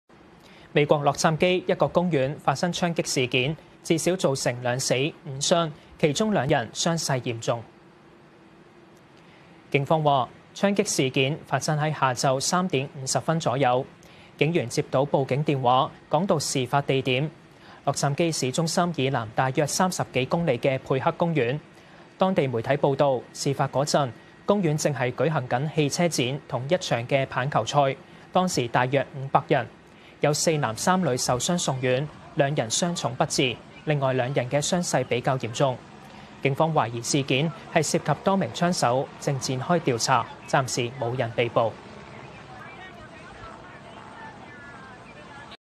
美國洛杉磯一公園發生槍擊 2死5傷｜香港V
美國洛杉磯一個公園發生槍擊事件，至少造成2死5傷，其中兩人傷勢嚴重。